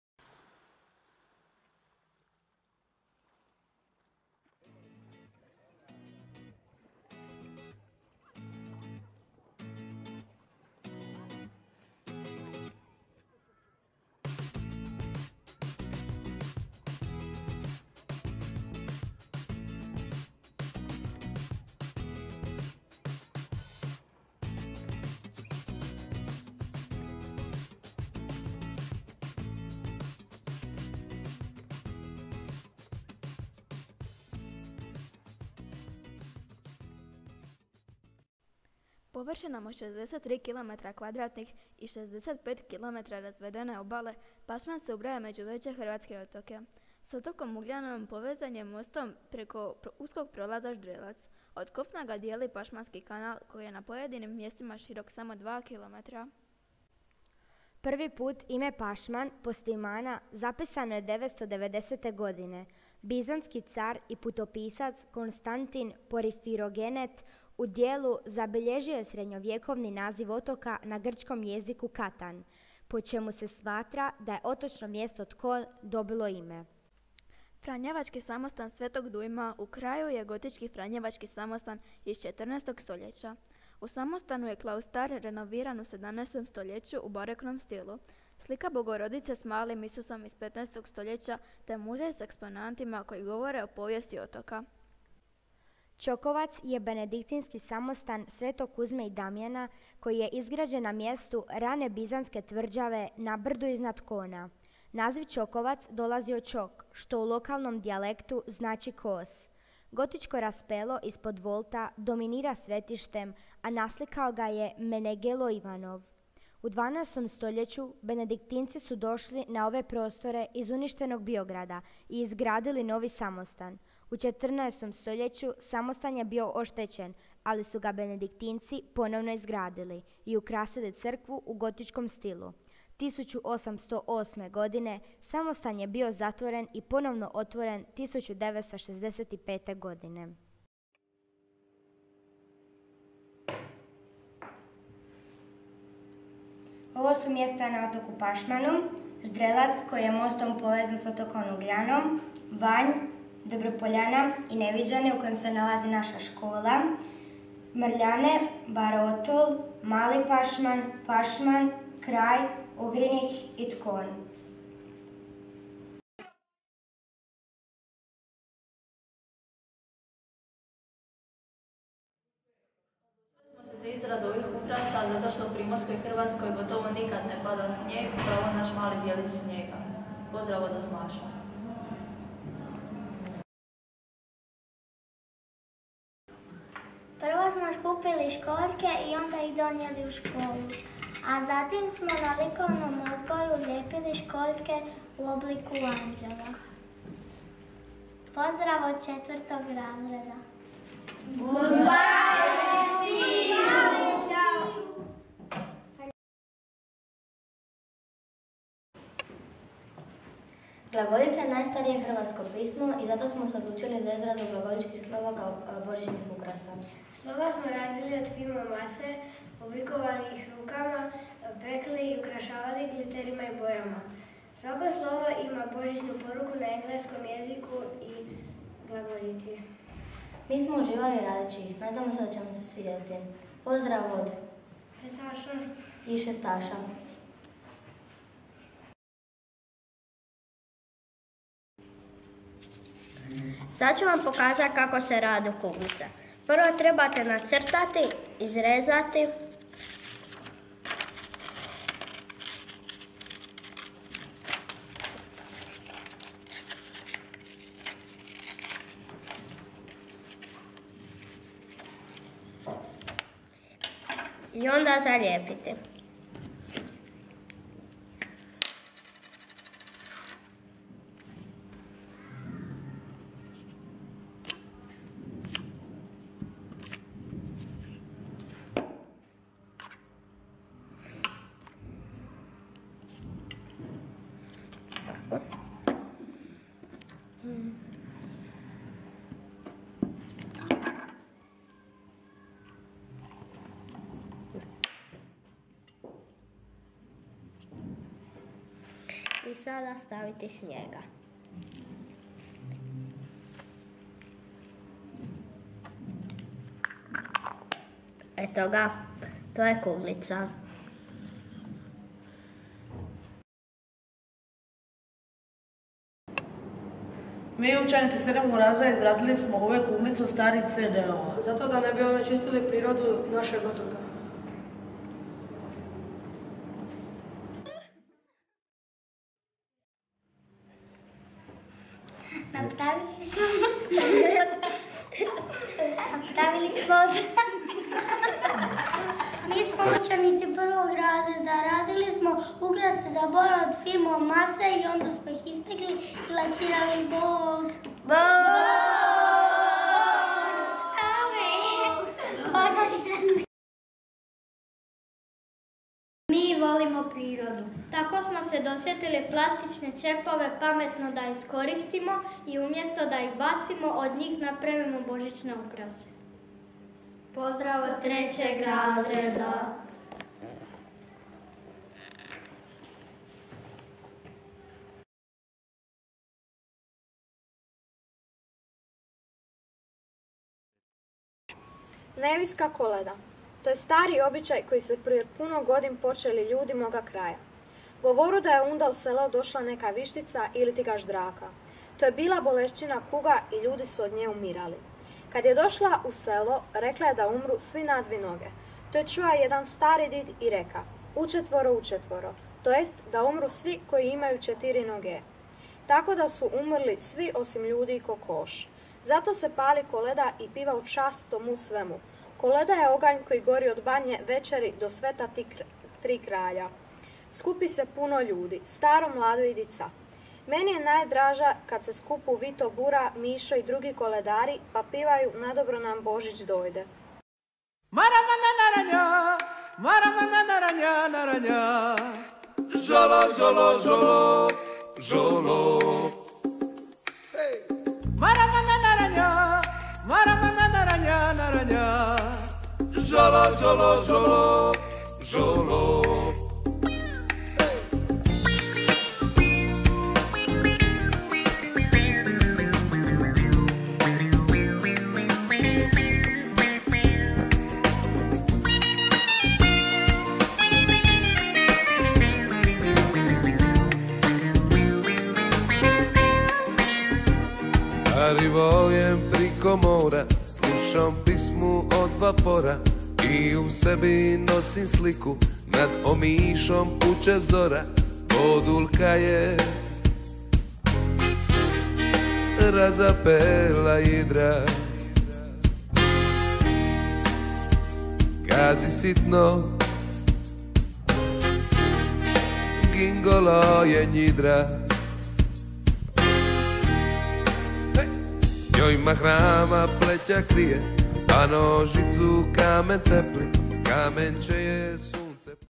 Christmast Tree Decoration Nevidjane, Croatia
Video of pupils from "Vladimir Nazor" School,  Neviđane.
They made christmas tree decorations and send them in other schools in Europe.